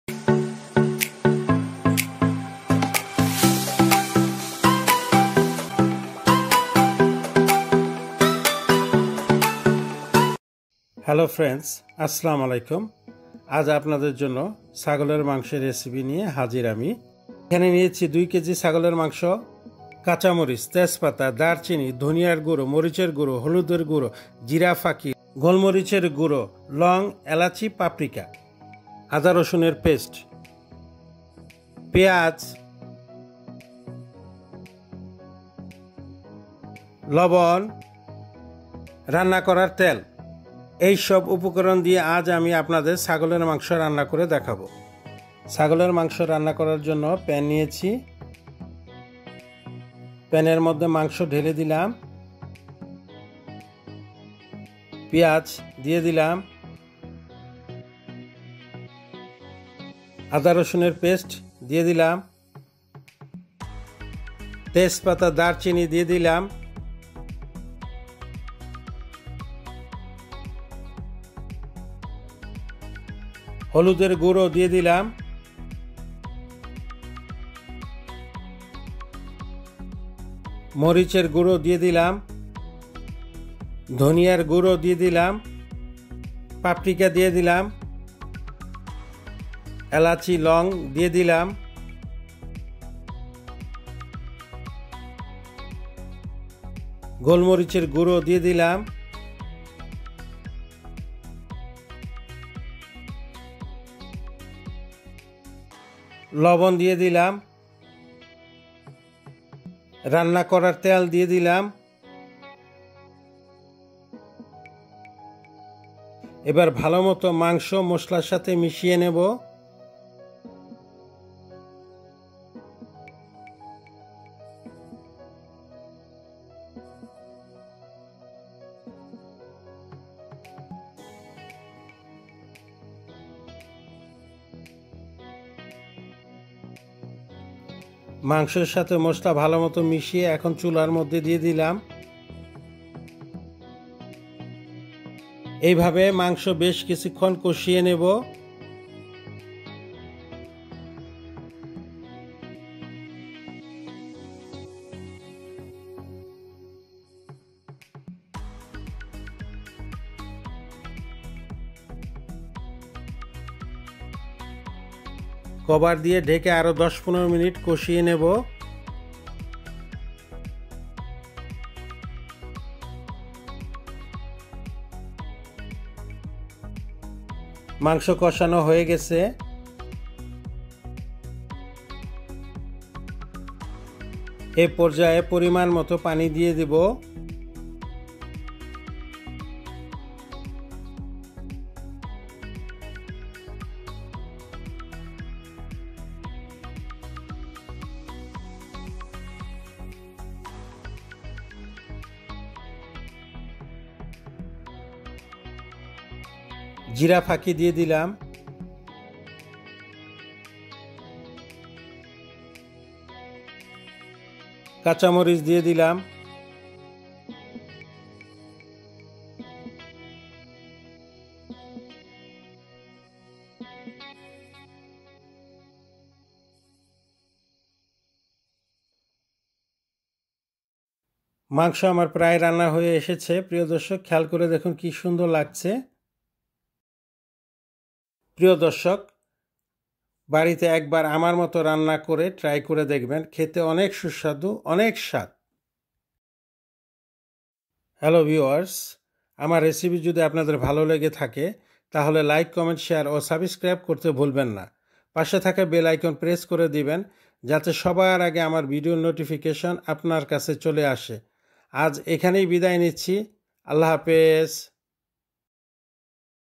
Goat Meat Recepi
(1)Goat Meat
(2)Pepper
(3)Bay Leaf
(4)Cinnamon
(5)Coriander Powder
(6)Chili Powder
(7)Turmeric Powder
(8)Cumin Powder
(9)Black Pepper
(10)Cloves
(11)Cardamon
(12)Parika
(13)Garlic Pest
(14)Ginger Pest
(15)Onion
(16)Salt
(17)Sunflower Oil